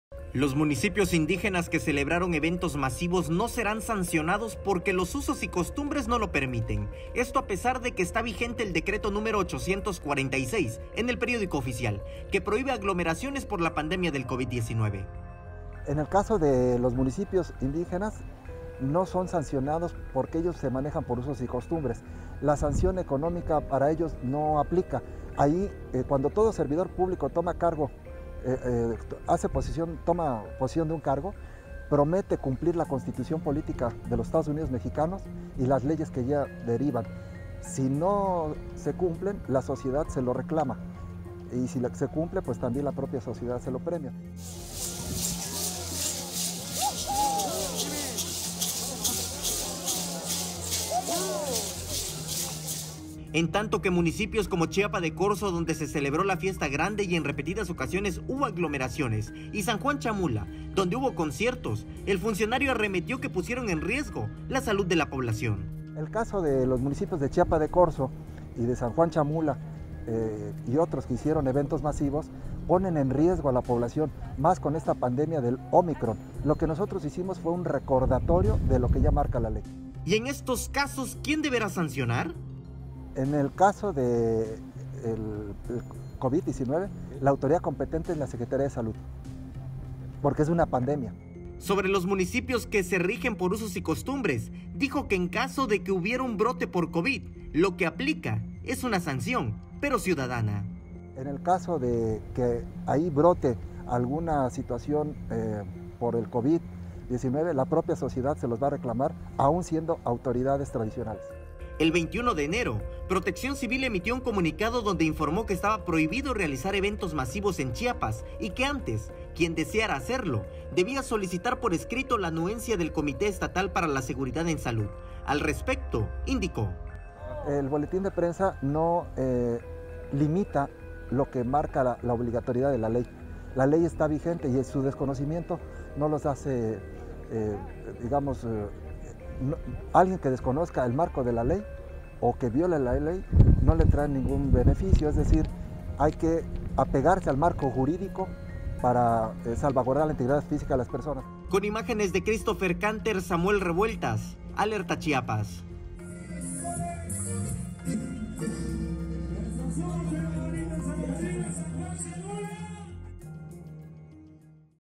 Usos y costumbres impiden sancionar a municipios que hicieron fiestas masivas: PC
Los municipios indígenas que celebraron eventos masivos no serán sancionados porque los usos y costumbres no lo permiten; esto a pesar de que está vigente el decreto número 846 en el Periódico Oficial, que prohibe aglomeraciones por la pandemia del COVID-19.